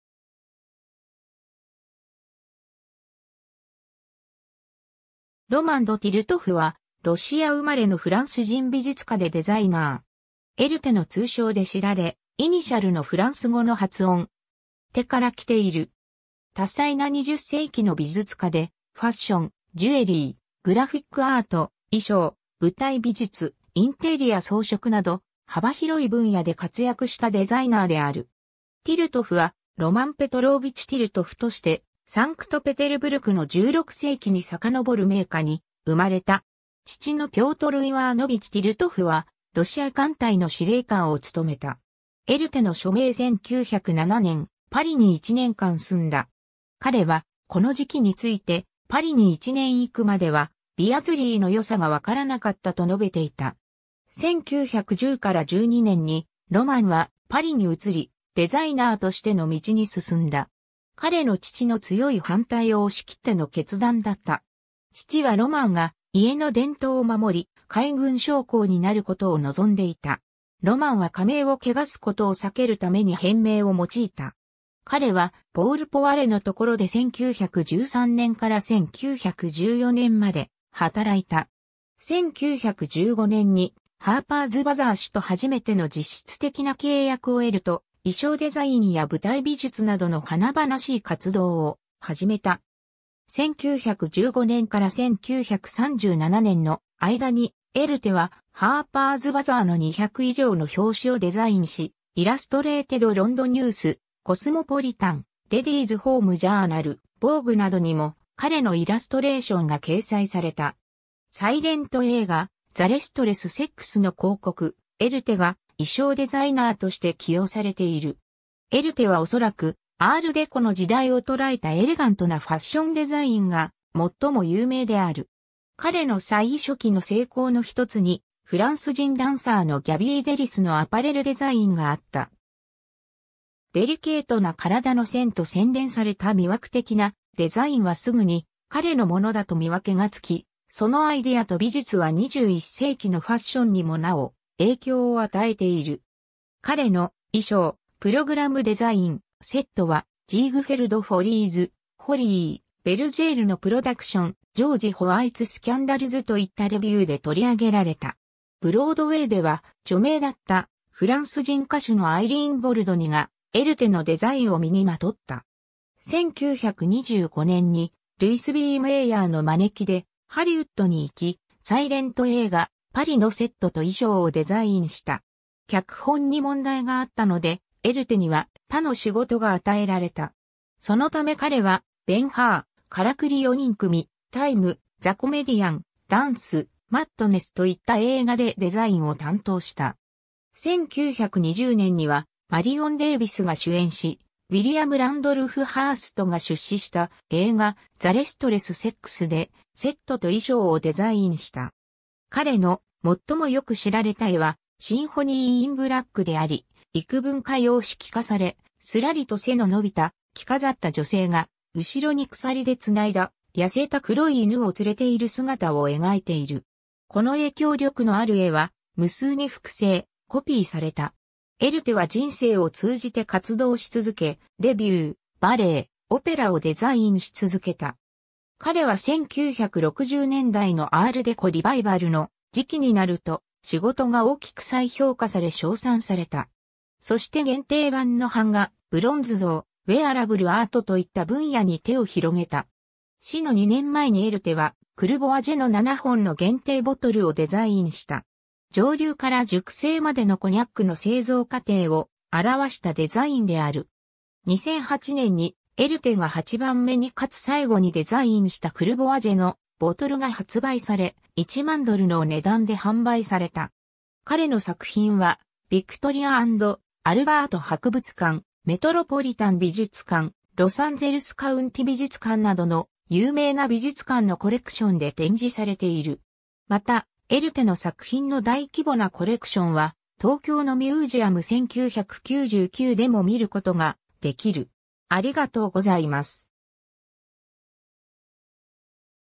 エルテ
ロマン・ド・ティルトフ（Romain de Tirtoff, 1892年11月23日 - 1990年4月21日）は、ロシア生まれのフランス人美術家でデザイナー。
エルテ（Erté）の通称で知られ、イニシャルのフランス語の発音 [ɛʁ.te] から来ている。
多才な20世紀の美術家で、ファッション、ジュエリー、グラフィック・アート、衣装、舞台美術、インテリア装飾など、幅広い分野で活躍したデザイナーである。
 ティルトフはロマン・ペトローヴィチ・ティルトフ（ロシア語: Рома́н Петро́вич Ты́ртов）として、サンクトペテルブルクの16世紀に遡る名家に生まれた。
父のピョートル・イワーノヴィチ・ティルトフはロシア艦隊の司令官を務めた。
 エルテの署名 1907年、パリに一年間住んだ。
彼はこの時期について「パリに一年行くまではビアズリーの良さが分からなかった」と述べていた。
 1910から12年に、ロマンはパリに移り、デザイナーとしての道に進んだ。
彼の父の強い反対を押し切っての決断だった。
父はロマンが家の伝統を守り、海軍将校になることを望んでいた。
ロマンは家名を汚すことを避けるために変名を用いた。
彼はポール・ポワレのところで1913年から1914年まで働いた。
 1915年に「ハーパーズ バザー」誌と初めての実質的な契約を得ると、衣装デザインや舞台美術などの華々しい活動を始めた。
 1915年から1937年の間に、エルテは「ハーパーズ バザー」の200以上の表紙をデザインし、「イラストレイテド・ロンドン・ニュース」、「COSMOPOLITAN」、「レディーズ・ホーム・ジャーナル」、ヴォーグなどにも彼のイラストレーションが掲載された。
 サイレント映画「The Restless Sex」の広告、エルテが衣装デザイナーとして起用されている。
 エルテはおそらく、アール・デコの時代を捉えたエレガントなファッション・デザインが最も有名である。
彼の最初期の成功の一つに、フランス人ダンサーのギャビー・デリスのアパレルデザインがあった。
デリケートな体の線と洗練された魅惑的なデザインはすぐに彼のものだと見分けがつき、そのアイディアと美術は21世紀のファッションにもなお影響を与えている。
 彼の、衣装、プログラム・デザイン、セットはジーグフェルド・フォリーズ（1923年）、フォリー・ベルジェールのプロダクション、ジョージ・ホワイツ・スキャンダルズといったレヴューで採り上げられた。
ブロードウェイでは、著名だったフランス人歌手のアイリーン・ボルドニがエルテのデザインを身に纏った。
 1925年にルイス・B・メイヤーの招きでハリウッドに行き、サイレント映画「巴里」のセットと衣装をデザインした。
脚本に問題があったので、エルテには他の仕事が与えられた。
そのため彼は、「ベン・ハー」、「からくり四人組」、「Time, the Comedian」、「Dance Madness」といった映画でデザインを担当した。
1920年には、マリオン・デイヴィスが主演しウィリアム・ランドルフ・ハーストが出資した映画「The Restless Sex」でセットと衣装をデザインした。
 彼の最もよく知られた絵は「Symphony in Black」であり、幾分か様式化され、すらりと背の伸びた着飾った女性が、後ろに鎖で繋いだ痩せた黒い犬を連れている姿を描いている。
この影響力のある絵は無数に複製・コピーされた。
 エルテは人生を通じて活動し続け、レヴュー、バレエ、オペラをデザインし続けた。
彼は1960年代のアール・デコ・リバイバルの時期になると、仕事が大きく再評価され称賛された。
そして限定版の版画、ブロンズ像、ウェアラブル・アートといった分野に手を広げた。
 死の二年前にエルテは、クルボアジェの七本の限定ボトルをデザインした。
蒸留から熟成までのコニャックの製造過程を表したデザインである。
 2008年に、エルテが八番目にかつ最後にデザインしたクルボアジェのボトル（1892年産のグランド・シャンパーニュのコニャックが入っていた）が発売され、1万ドルの値段で販売された。
 彼の作品は、ヴィクトリア&アルバート博物館、メトロポリタン美術館、ロサンゼルス・カウンティ美術館などの、有名な美術館のコレクションで展示されている。
またエルテの作品の大規模なコレクションは、東京のミュージア...